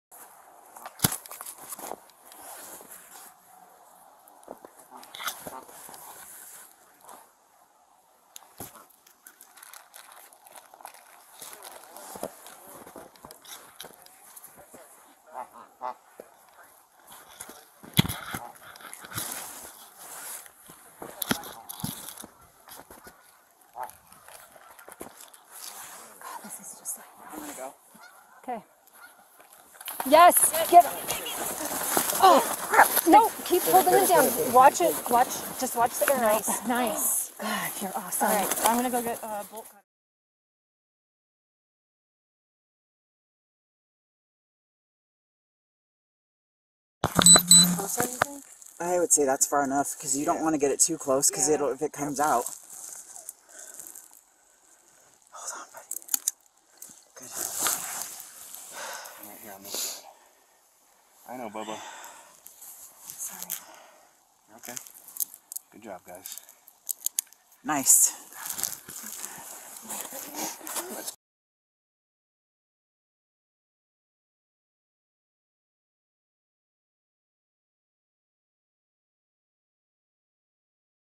Officers rescue goose with arrow lodged in body
Police on Wednesday released video of the animal's rescue in Colorado's Boulder to help find those responsible for shooting a goose with an arrow, leaving it lodged in its body near its beak. The video shows animal control officers using a net to capture the goose and then cutting off most of the arrow before taking it in for medical attention.